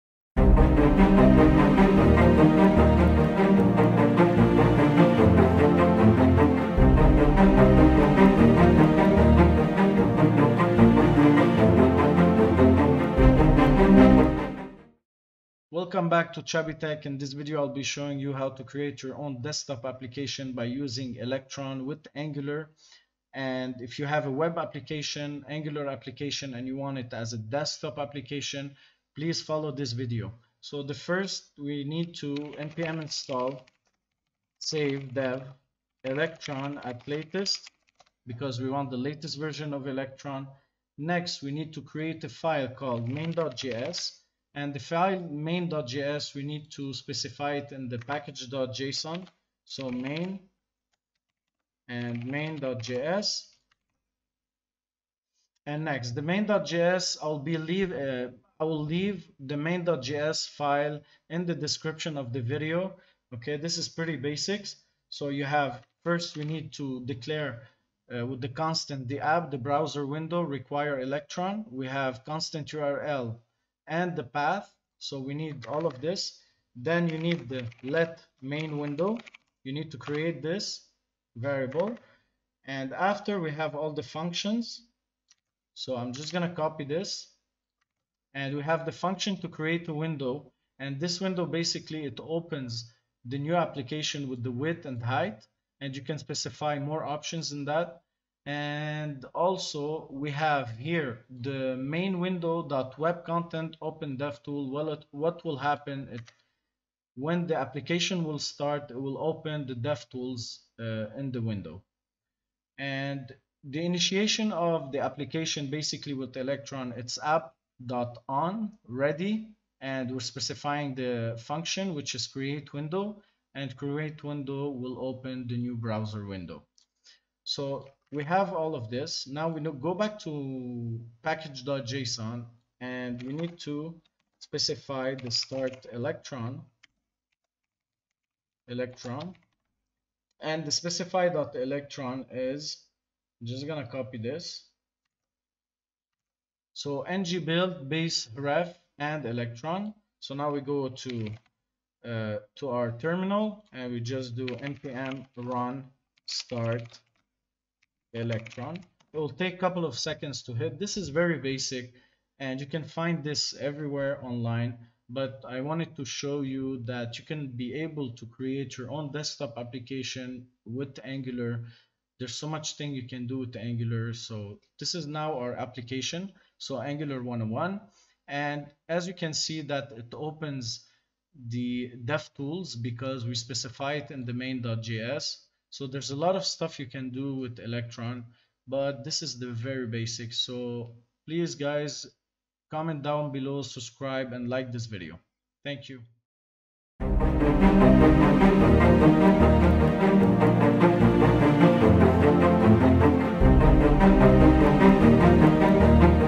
Create an Electron App with Angular: Feel Like a Pro Developer
Learn how to build an Electron app with Angular in this tutorial! With the power of these two tools, you can create a cross-platform desktop application. Watch now to get started!